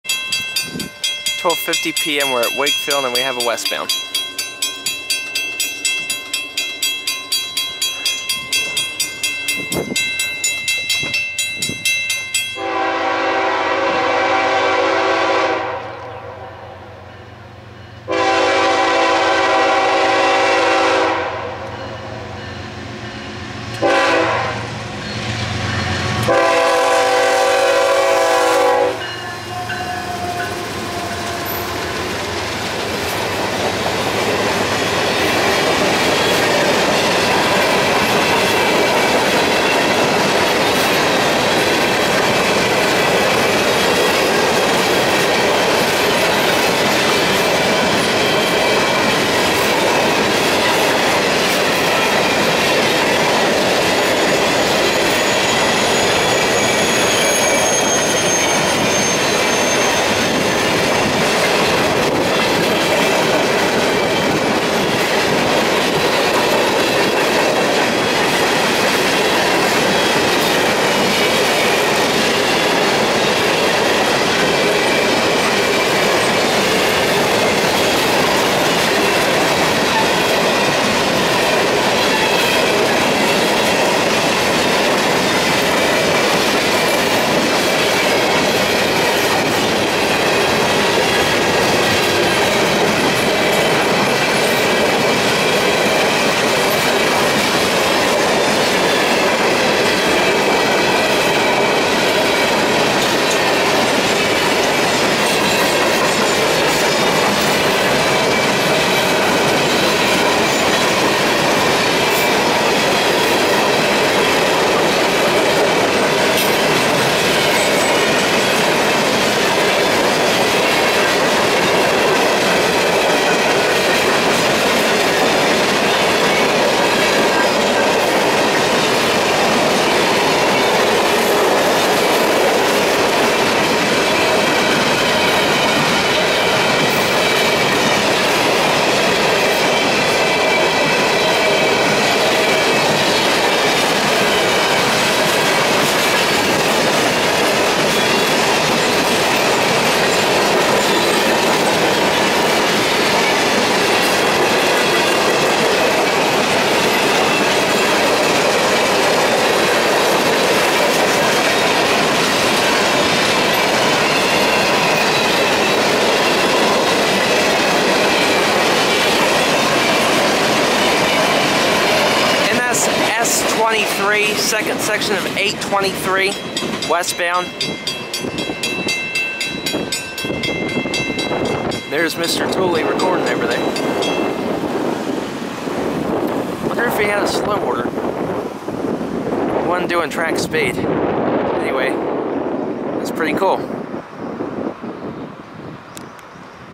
NS S23
12:50 PM

Nearly three long hours passed before NS ran anything else. This was the first of two three hour lulls, in addition to two two hour lulls I would experience that day.

NS S23, second section of unit empty coal train 823, rolls west at a restricted speed with an ES44AC and a Tophat for power. The lead unit is #8110 and is almost brand new. Awesome K5HLL horn on the lead unit. Mostly Top Gons in the consist, but a few others mixed in as well. David Tooley, Jason Underwood, and Mr. Tooley (David's dad) had showed up by this point to enjoy NS.

Recorded 5.1.2012